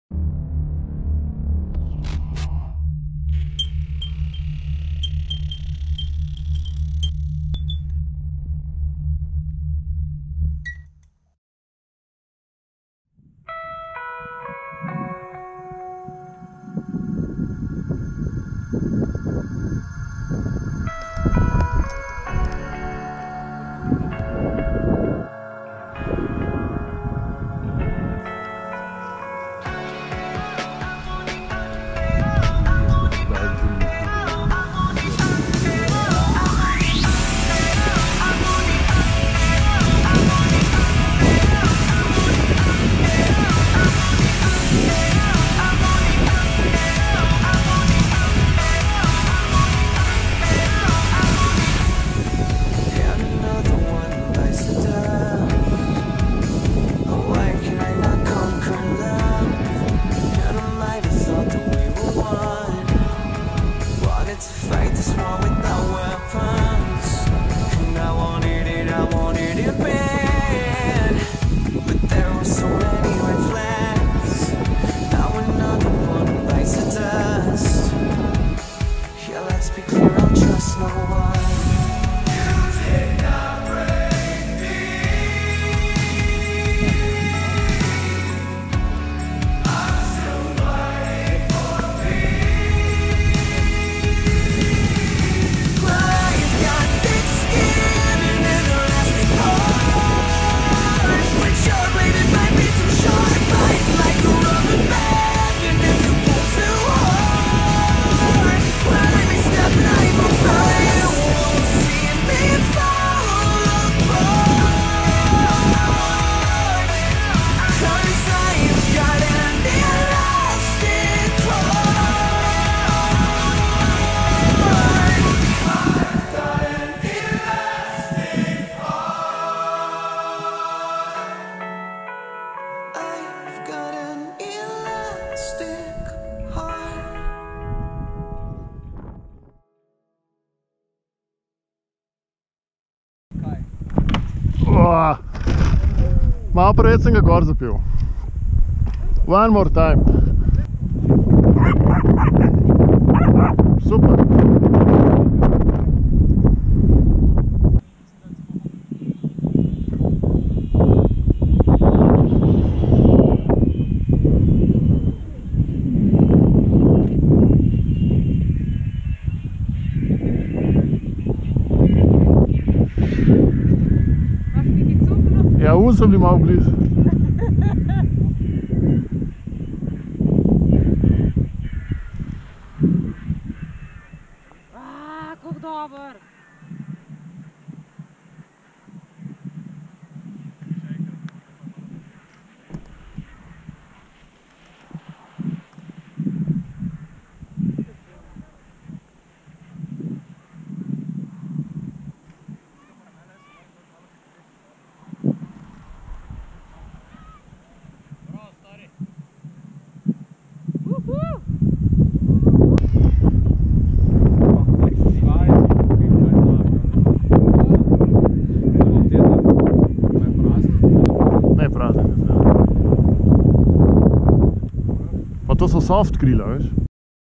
Mibo ASW15B on Nanos
It was terrific day on Nanos /Slovenia. Thermal and low wind. Ideal for high speed sends with the new Mibomodeli ASW15B with 5,5kg.